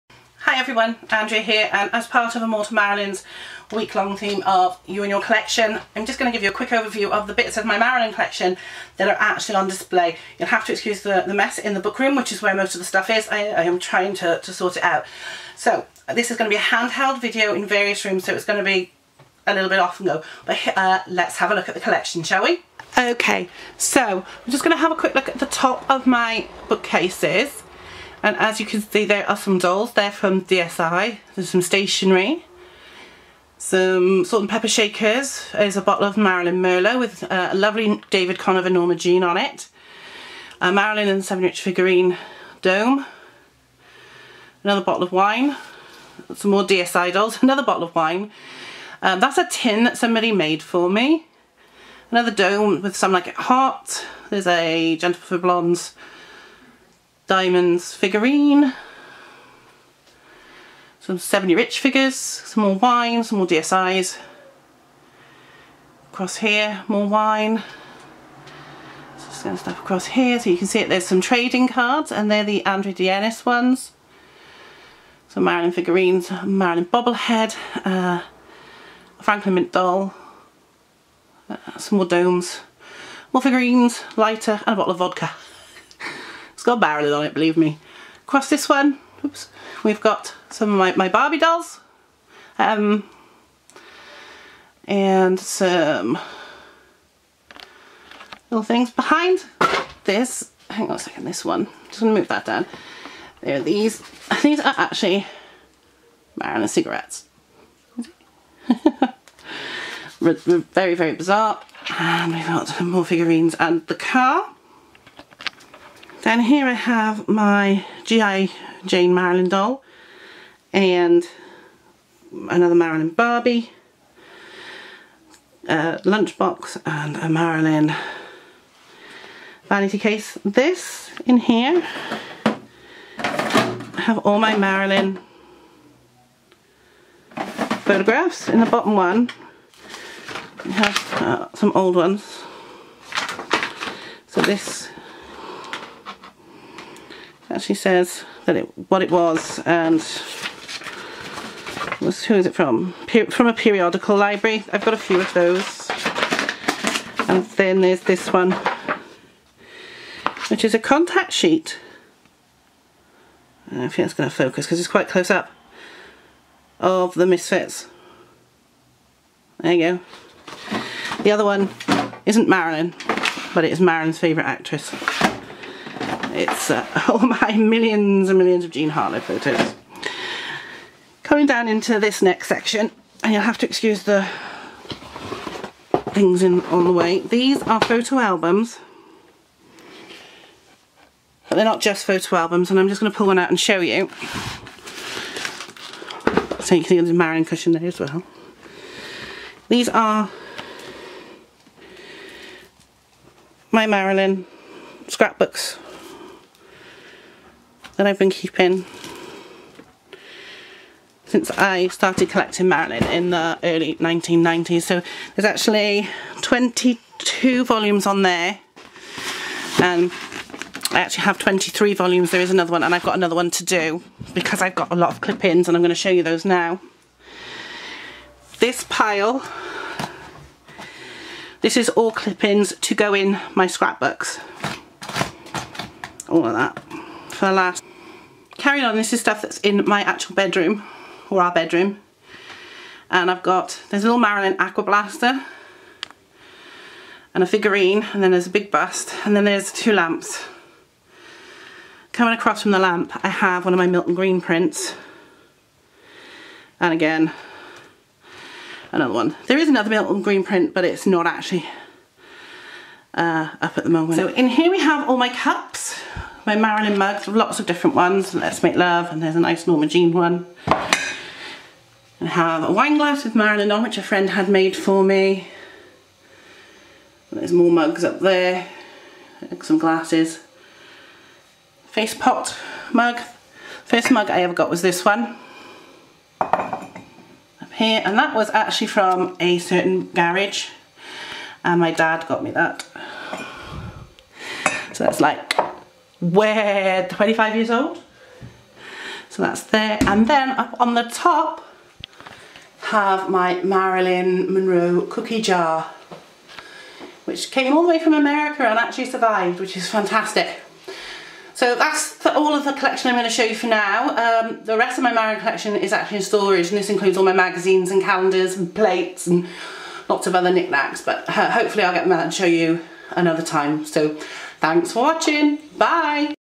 Marilyn Monroe collection
This is a very small part of my Marilyn Monroe collection - a lot is currently in storage - hopefully I will be able to share more of it with you soon